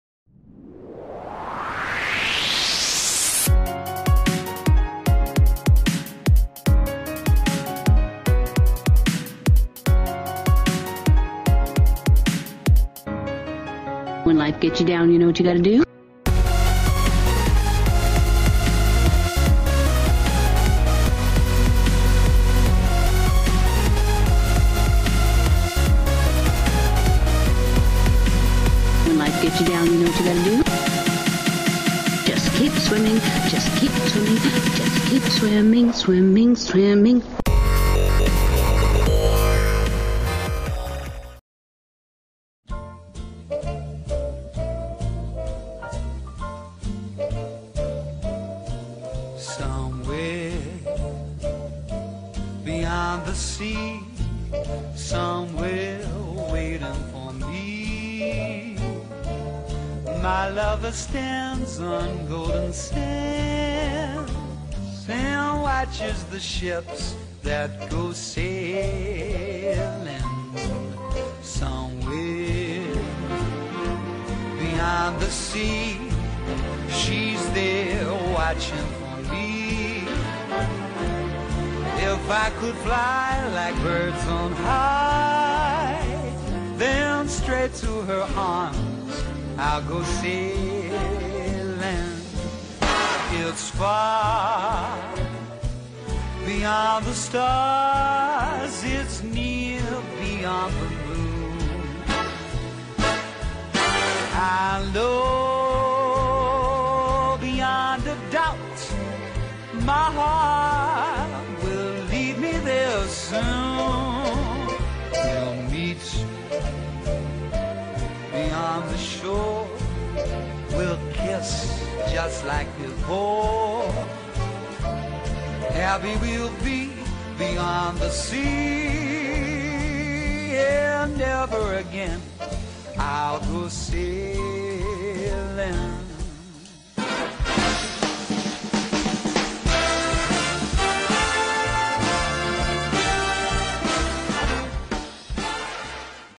Finding nemo/dory makeup
Did a inspired makeup look from finding nemo amd dory